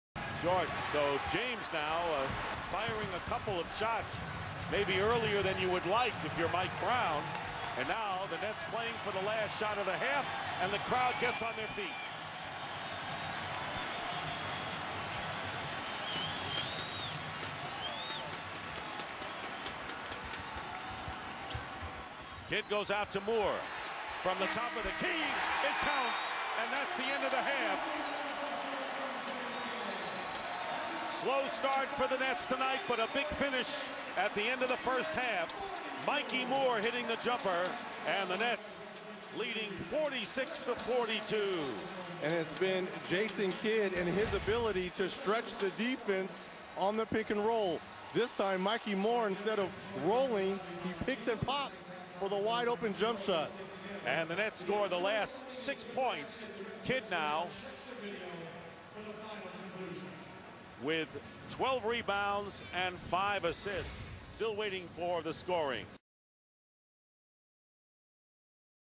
Mikki Moore beats the halftime buzzer with the long 2
Assist by Jason Kidd.
May 14, 2007. Game 4 of the Eastern Conference Semifinals between the New Jersey Nets & the Cleveland Cavaliers.